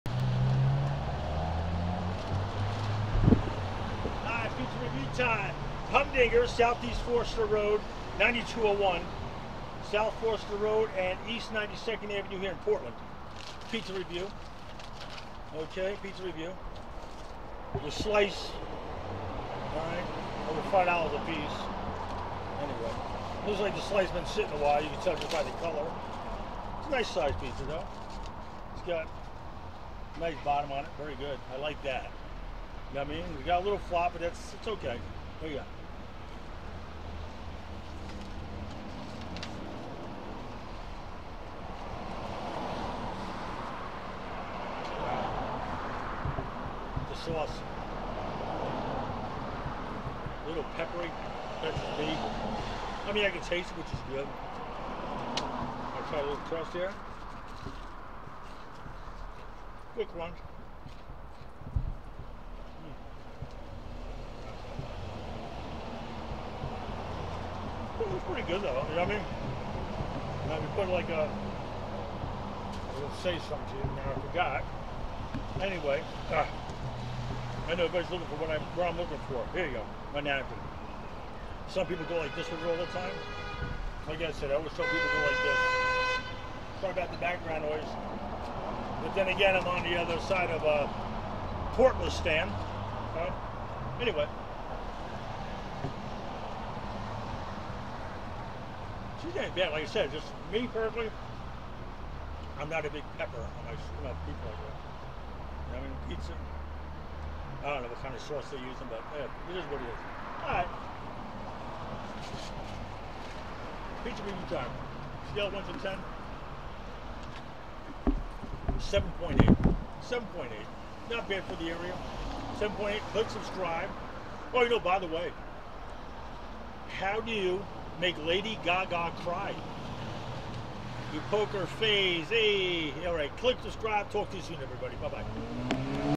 Humdinger Pizza 9201 S .Foster Rd & 92nd Ave Portland Oregon
Pizza Review Scale 1-10 A 7.8 Friendly service nice staff. For the pizza typical flour recipe. Sauce was way loaded with pepper nice crunch a little greasy but it is what it is..